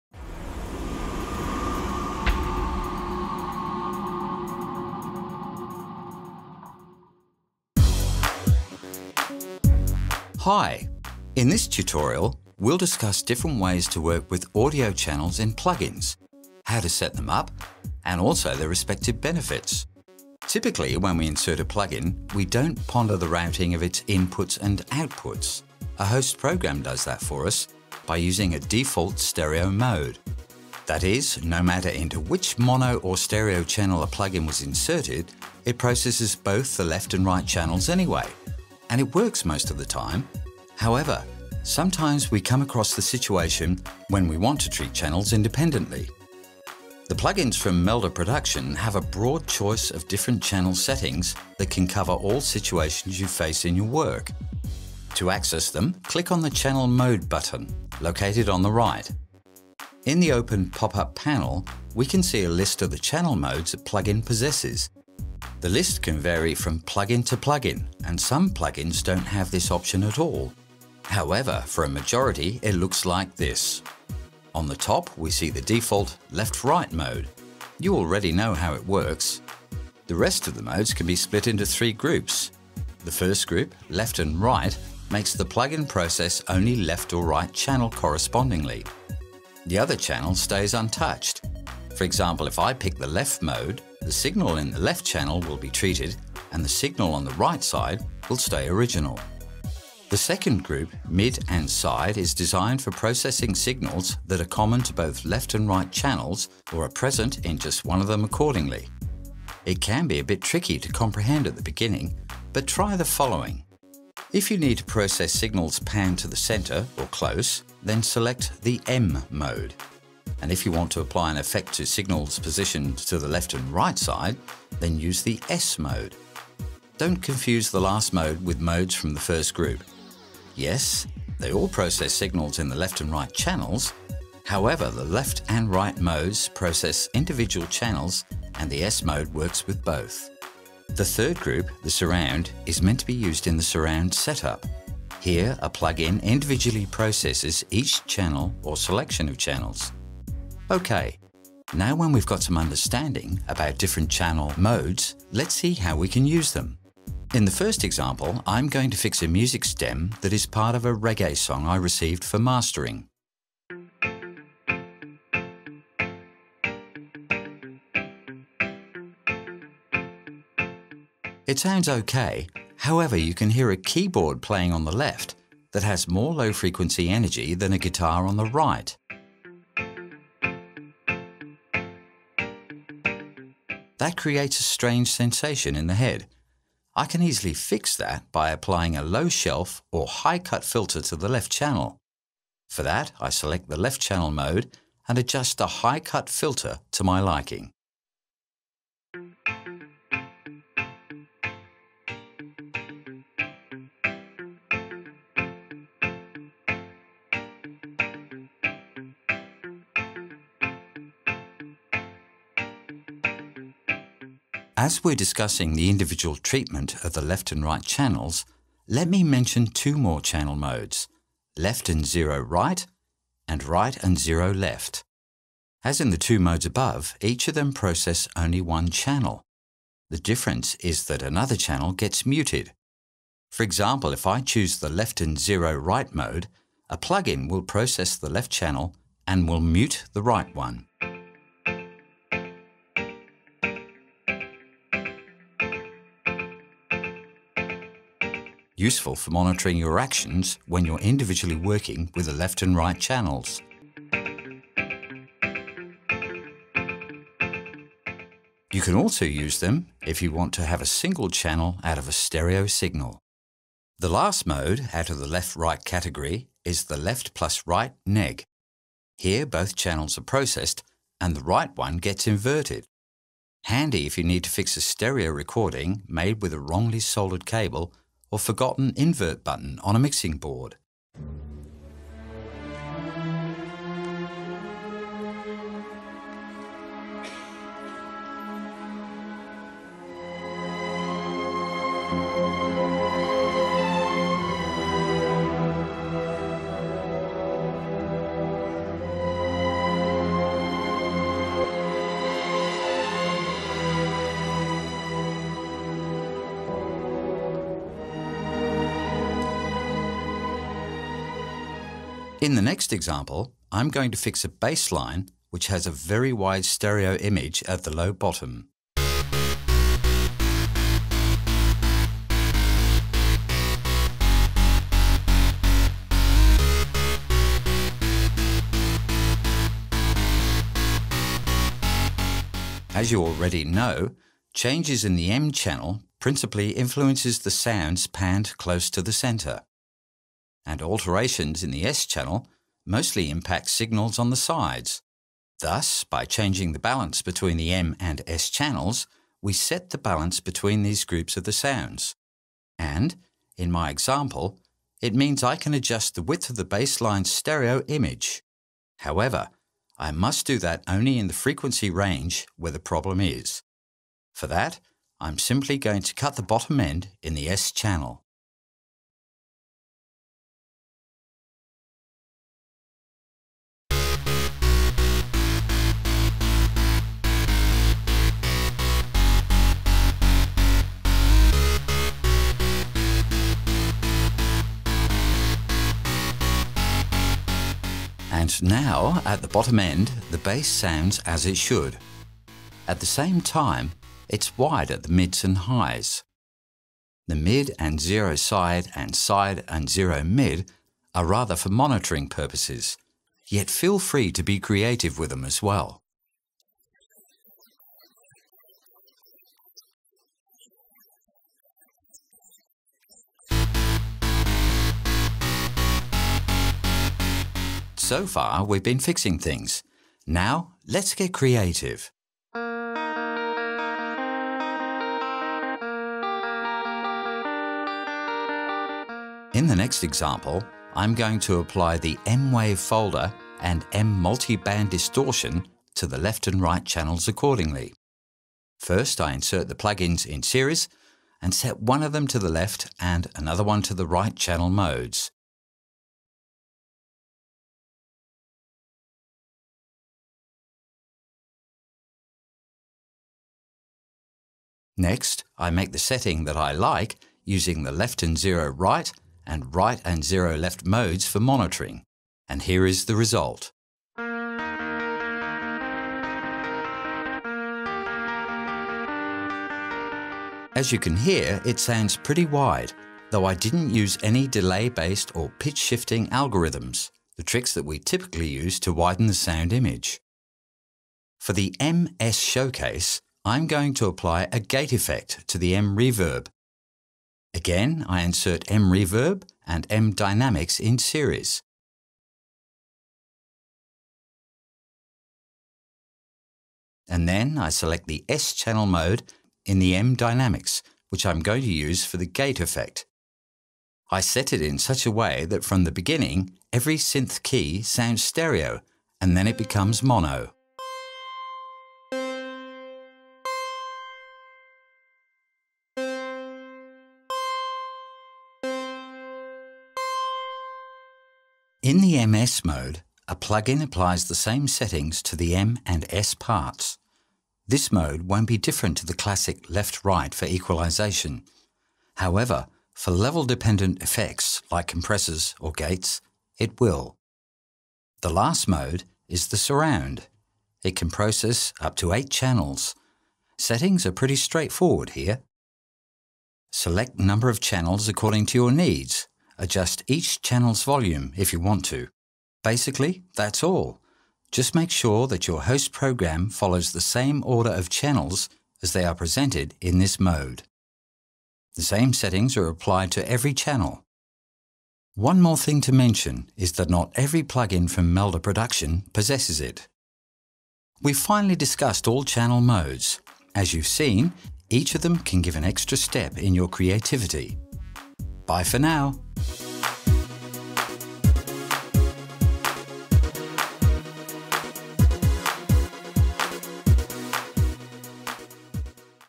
Left/right, mid/side, surround... audio processing
This tutorial explains how to use mid/side. surround, single channel and other channel processing modes and describes a few useful techniques.